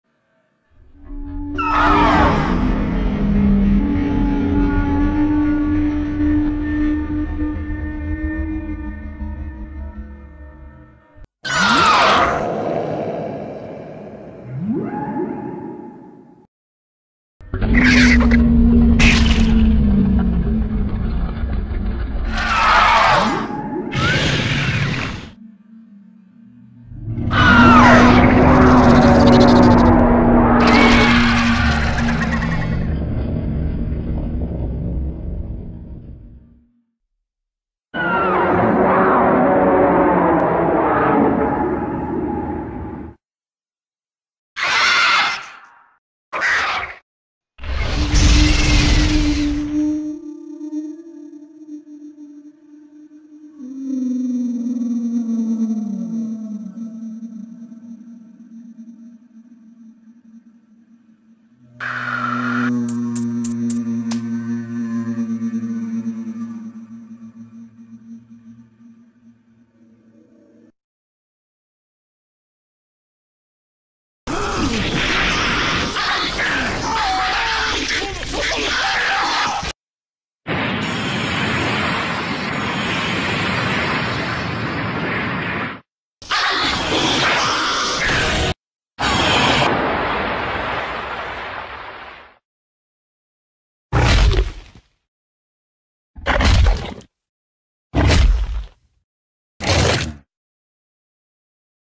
Titanus Thomas The Kaiju Engine Sounds
Titanus Thomas The Kaiju Engine. New Kaiju from before Monsters saw everyone o kaijus Male. Titanus Percy and Titanus James. but 2020 Heroe convert to Kaiju

Trivia:
This Kaiju Like Similar to Labyrinthodont from Primeval

War Of The Monsters: Rise (2023)

Requested by @andrebarros5946